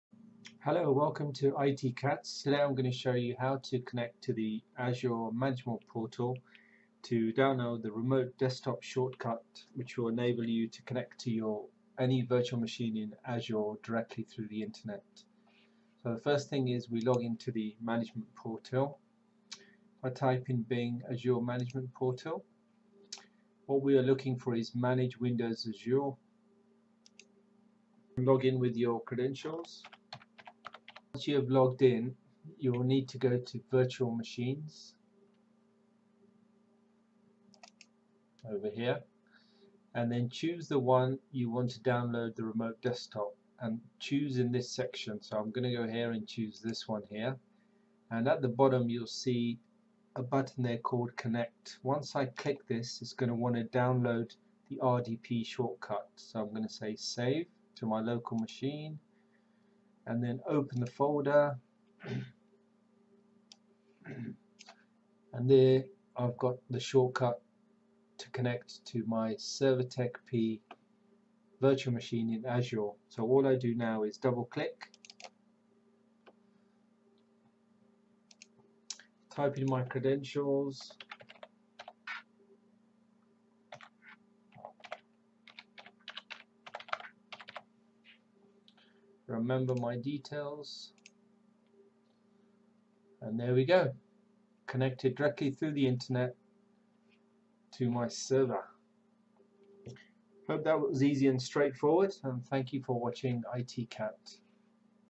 How to connect to a Virtual Machine in Microsoft Azure using RDP
I will show you how to RDP / connect to a virtual machine that you have created within Microsoft Azure.